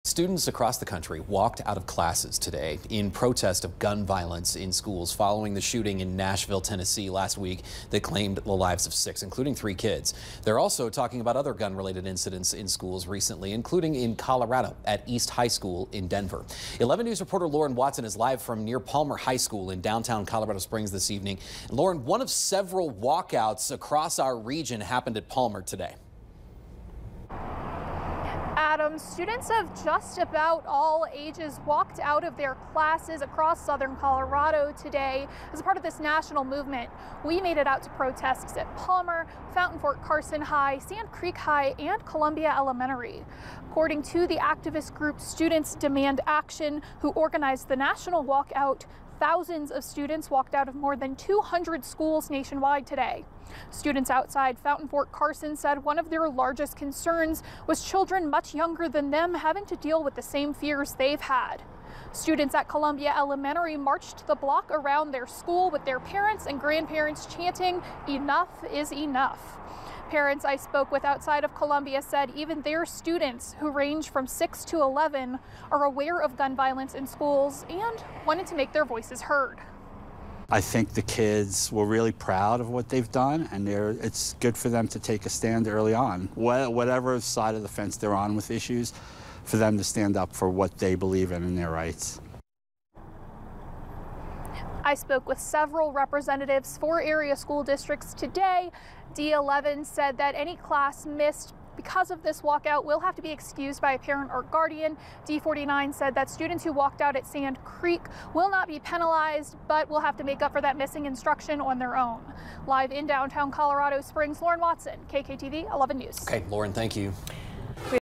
WATCH: Southern Colorado students walk out to protest gun violence in schools
Students at a number of schools in southern Colorado walked out of their classes Wednesday.